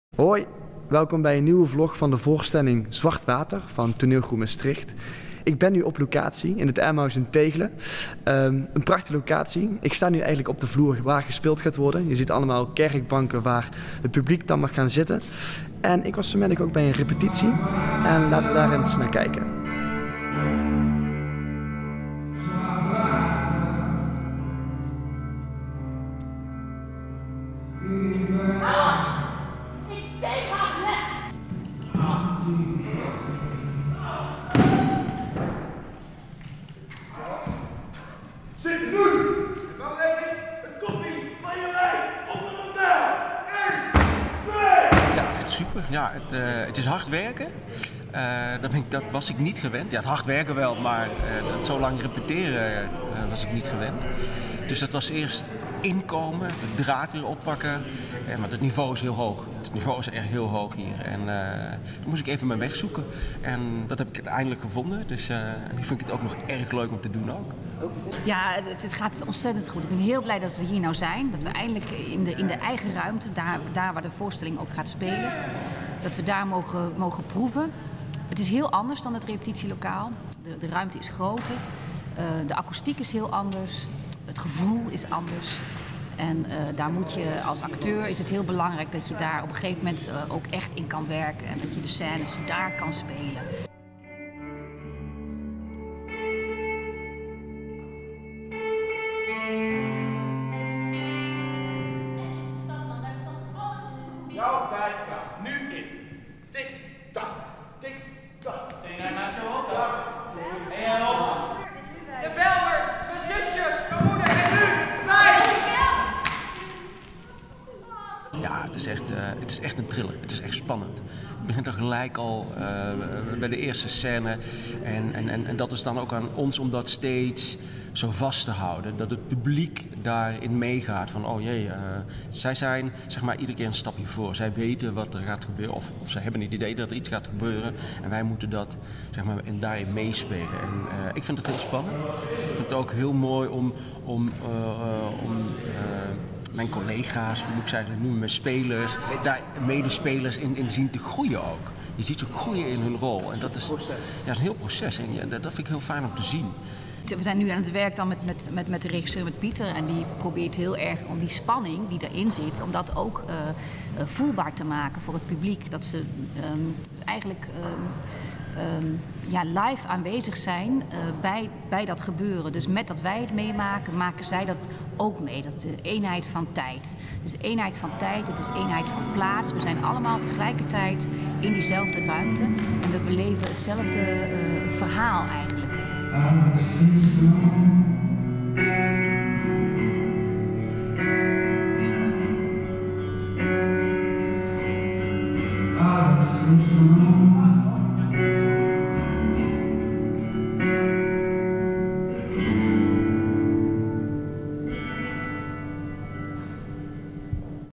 JOOP VLOGT | Zwart Water #2
JOOP VLOGT! Joop volgt de repetities en voorbereiding van Zwart water | Toneelgroep Maastricht. In deze vlog zijn hoofdrolspelers Abbie Chalgoum en Judith Pol aan het woord en krijgen we een sneak peak van de eerste repetities op locatie bij Emmaus-Feniks Tegelen - gem. Venlo.

Abbie en Judith spelen samen met acteurs en actrices van Toneelacademie Maastricht: Renee de Graaff, Tim Helderman, Mark Lindeman, Liza Macedo Dos Santos, Tarik Moree en Nathalie Roux.

In de toneelthriller 'Zwart Water' volgen we het verhaal van David. Na een uit de hand gelopen overval op een campingwinkel wordt hij, ver weg van zijn vrienden en Venlo, in een zorgboerderij geplaatst. Hij maakt een nieuwe start bij een nieuw gezin en ontmoet daar Nina, een jong meisje dat hem begrijpt. Totdat zijn oude vrienden van de overval hem vinden en de hel losbreekt. Onder dreiging van geweld moet David kiezen: voor zijn nieuwe leven of voor dat waar hij vandaan komt.

Morgenavond zijn regisseur Pieter Kuijpers, Abbie, tekstschrijver Frans Pollux en Peter Beeker te gast in Nedinscoplein, om 17:30 uur te zien op Omroep Venlo.